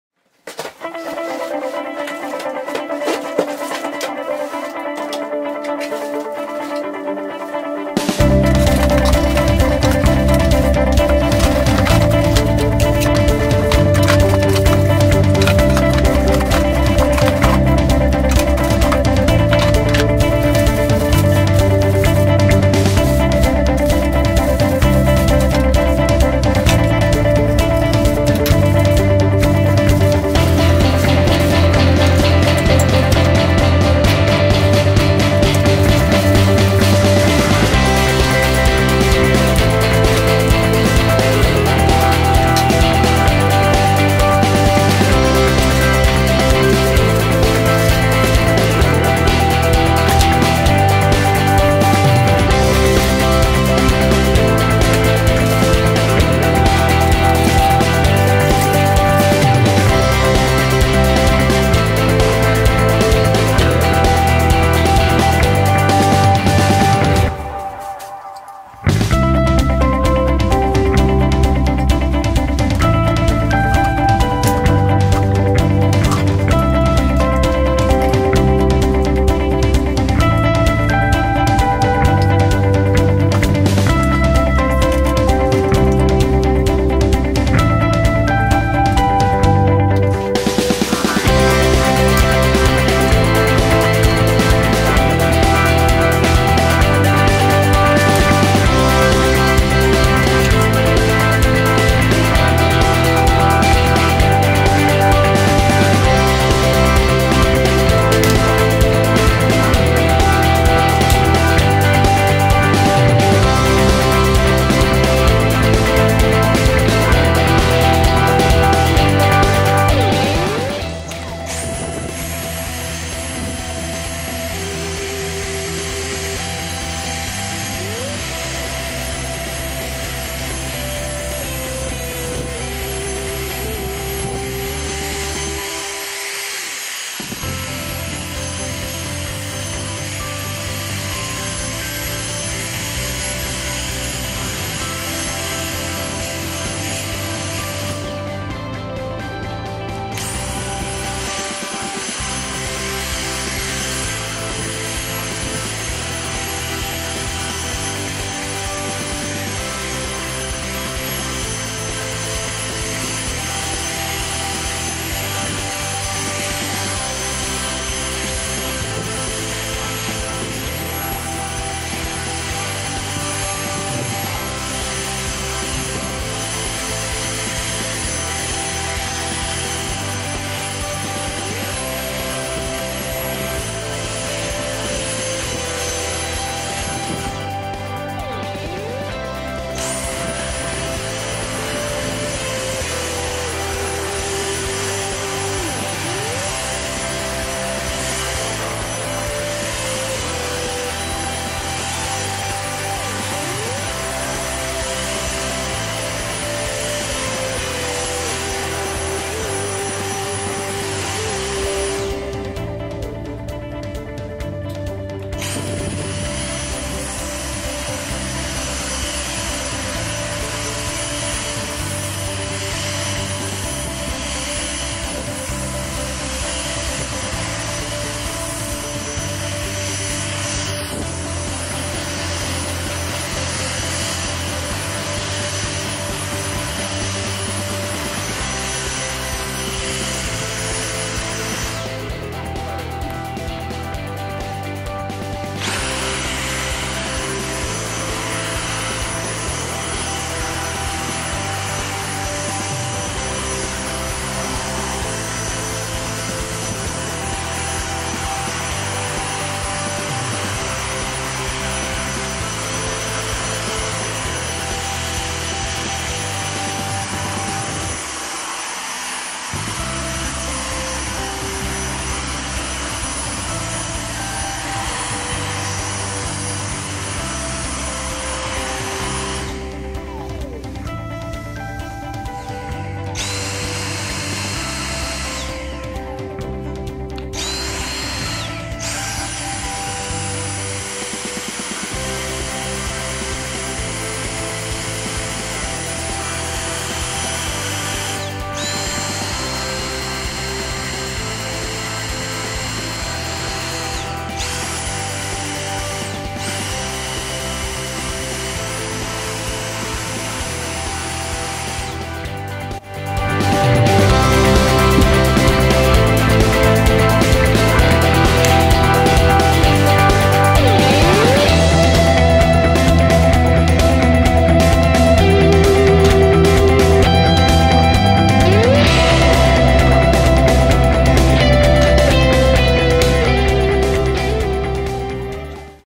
Dyson DC59 Cordless Vacuum Cleaner Unboxing and Testing
This is the unboxing, assembling and testing video of the new Dyson DC59 Animal Cordless Vacuum.